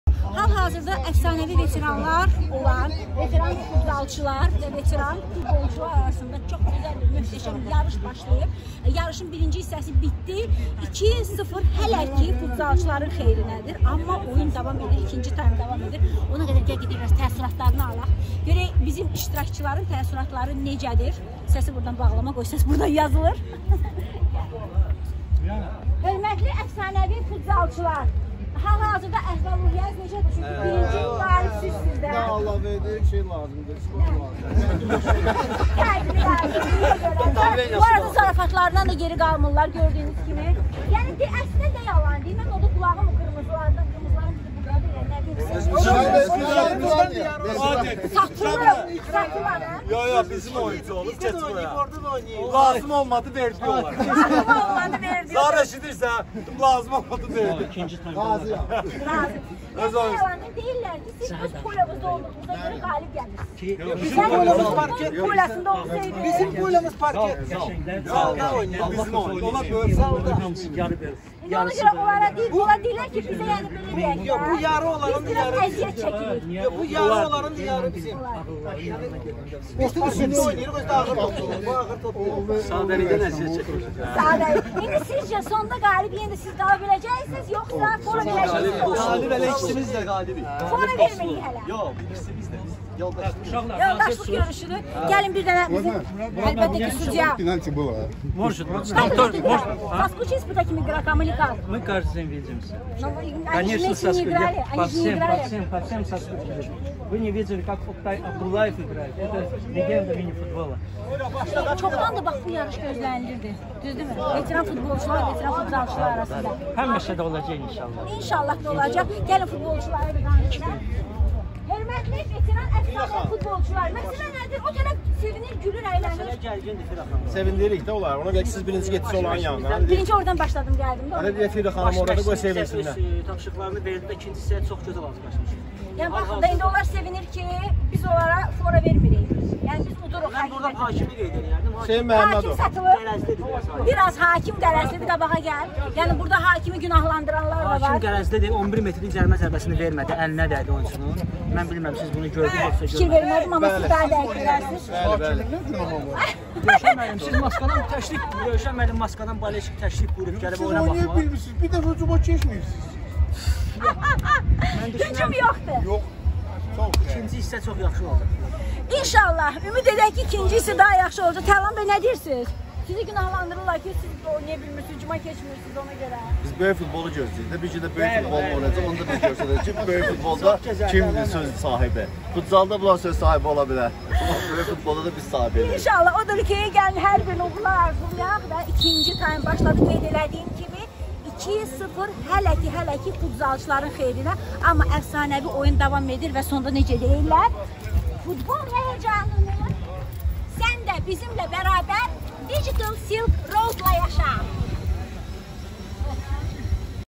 Gözlənilən oyun “Əfsanəvi Veteranlar” 🏆⚽️ FUTBOLÇULAR VS FUTZALÇILAR
Bu Videoda əfsanəvi Veteran futbolçularla əfsanəvi veteran futzalçılar bir biri ilə futbol matçı edirlər. Daha ətraflı məlumat üçün gözünüz DSR TV kanalında olsun.
“Əfsanəvi Veteranlar” 🏆⚽️ by @digitalsilkroad

Bütün iştirakçılar oyundan öncə PCR (Covid-19) testindən keçərək neqativ cavab almışdılar.

📌 Veteran futbolçular ilə veteran futzalçılar arasında çoxdandır gözlənilən dost oyunu.

📌 Təşkilatçı: @digitalsilkroad 💪

DİQQƏT!
Bu kanal rəsmi olduğu üçün kanal sahibinə  məxsus olan bütün videoların müəllif hüquqları Digital Silk Road şirkəti tərəfindən qorunur.